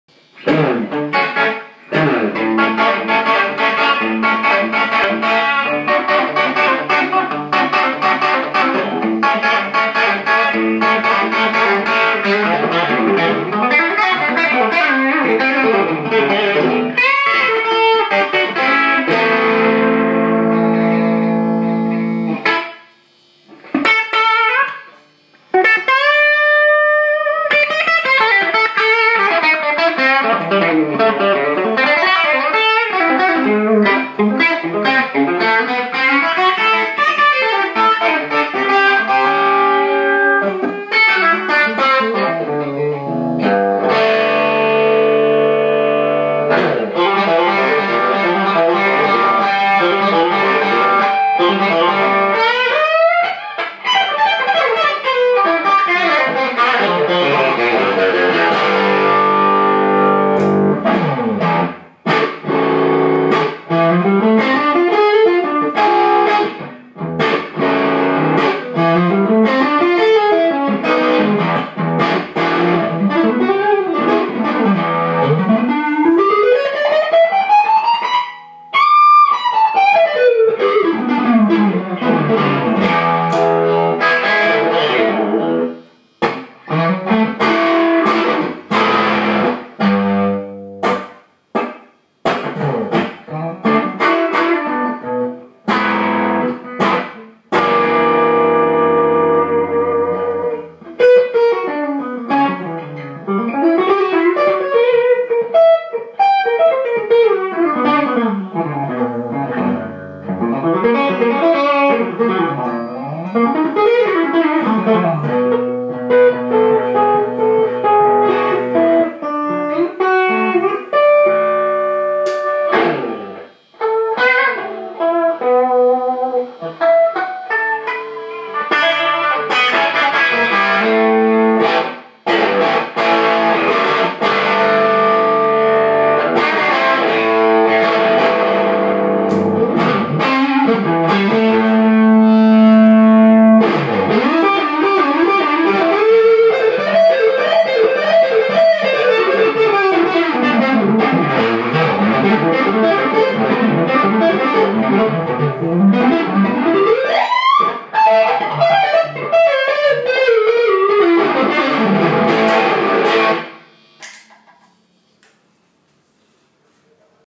Gigliotti GT Custom 2004 - Fralin Blues - Ethos Overdrive - '64 Blackface Bandmaster - Andre Dinuth
Andre Dinuth plays Gigglioti GT Custom 2004 with Fralin Blues tele pickups,  thru Ethos Overdrive, into a 1964 Blackface Fender Bandmaster Head. Speaker is 1x12 Celestion (unknown type) in a homemade sealed ported cabinet. Sorry for the poor video & audio quality (captured with iphone).

Hope you still enjoy!